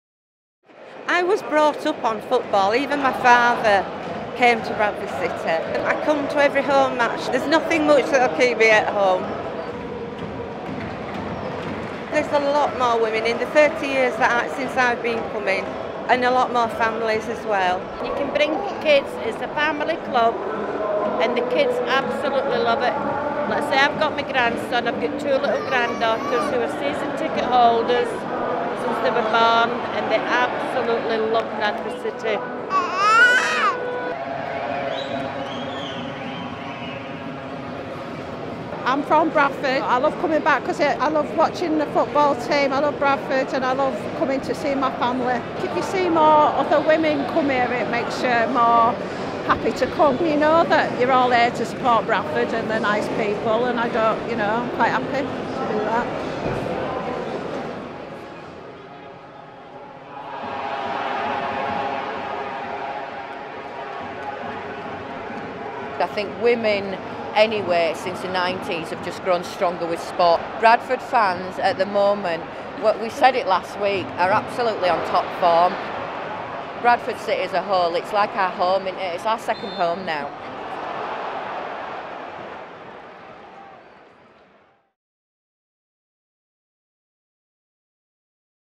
City Girls exhibition at the National Science and Media Museum, Bradford
The women of Bradford City talk about what football and the club mean to them.

City Girls is an exhibition of Nudrat Afza's photographs of female Bradford City fans. 17 November 2017 – 24 June 2018, National Science and Media Museum, Bradford, free entry.